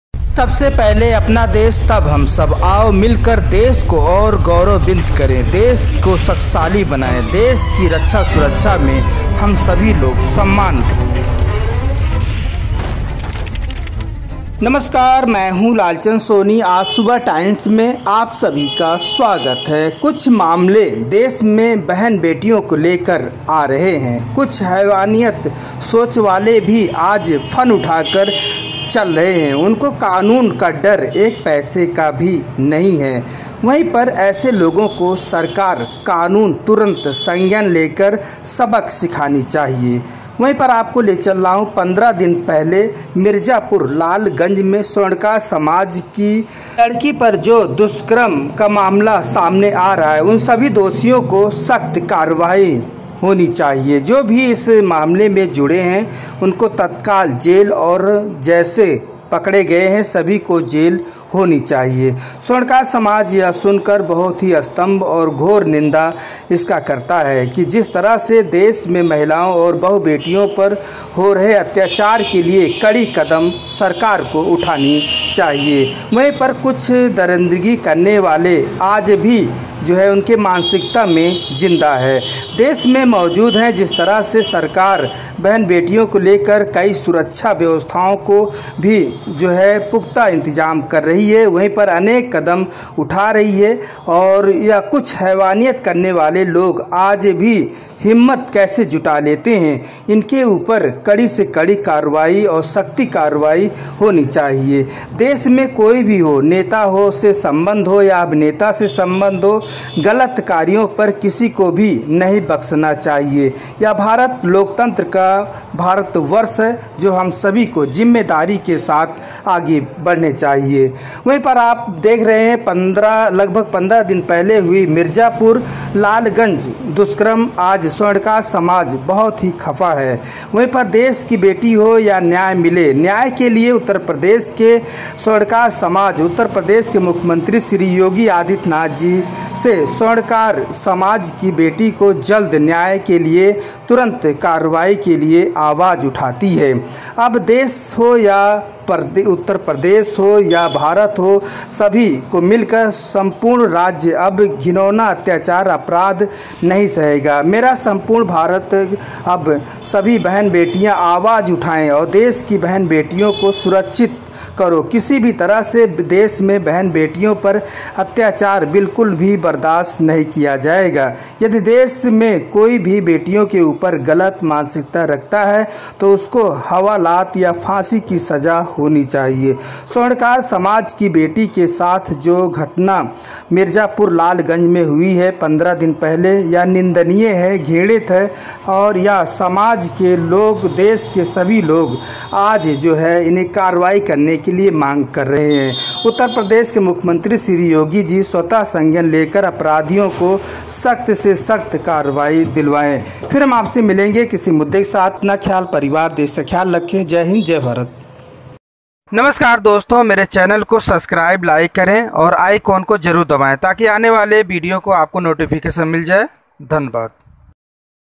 मिर्जापुर लालगंज मे स्वर्णकार समाज़ की बच्ची से घिनोऊना हरकत देश की बेटी न्याय मांगे घटना निंदनीय है
उ प्रदेश मिर्जापुर लालगंज मे नाबालिक स्वर्णकार समाज़ की बच्ची से घिनोऊना हरकत निंदनीय
स्वर्णकार समाज की बेटी के साथ हुई घटना निंदनीय है इसका स्वर्णकार सेवा समाज घोर निंदा करता है उत्तर प्रदेश के मुख्यमंत्री श्री योगी आदित्यनाथ जी से सभी स्वर्णकार समाज़ के लोग अनुरोध कर रहे है कि अपने सज्ञान में लेकर दोशियो को सख़्त से सख़्त सज़ा देश की बेटी को न्याय मिल सके यह घटना बताया जा रहा है कि 15 दिन पहले का है लेकिन अब यह संदेश शासन प्रशासन तक पहुँचते ही घटना की कार्यवाही देखी जा रही है करीब 15 दिन पहले की है मिर्ज़ापुर स्वर्णकार समाज से वार्ता हुई हमसे सभी दोषी गिरफ़्तार हो गए हैं वहा के स्वर्णकार जिला अध्यक्ष श्री अमरनाथ सेठ ने समाज की बेटी को न्याय दिलाया
जय स्वर्णकार समाज